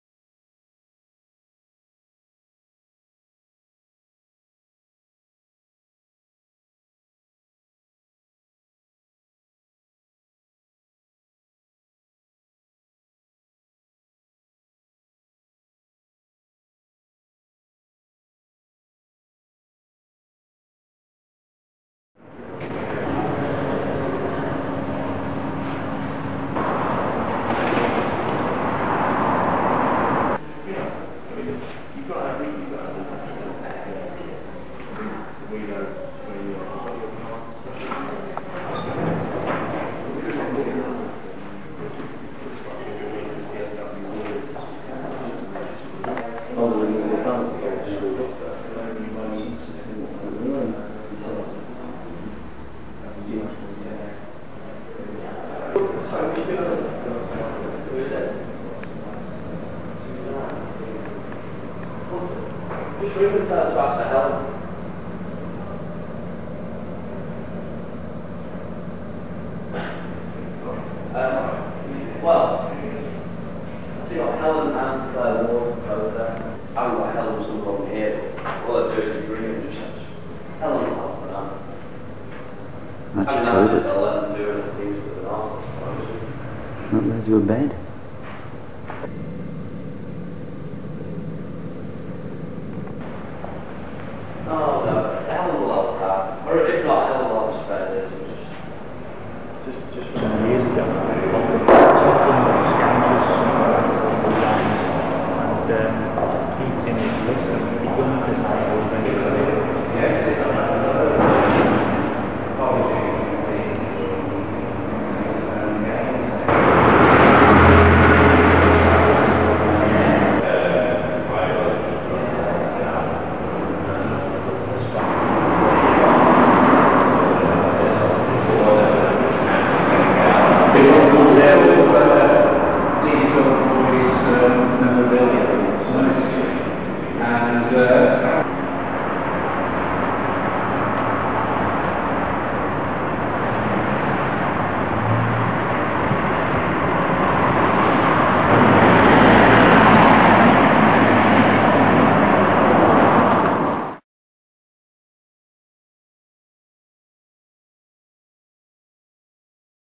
Liverpool Collegiate School re-opening 2000
It's no longer the school but it's still the school.
The official opening day.